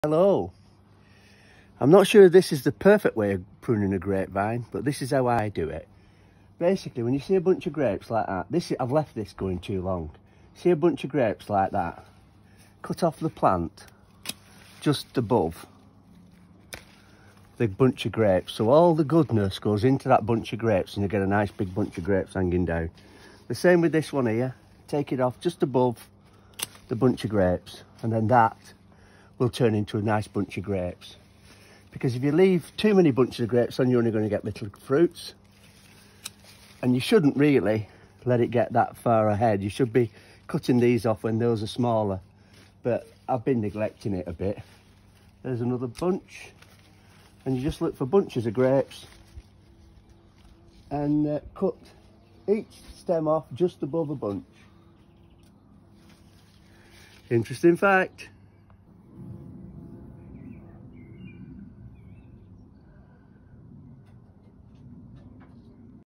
How to prune a grape vine
How to prune a out door grape vine in the UK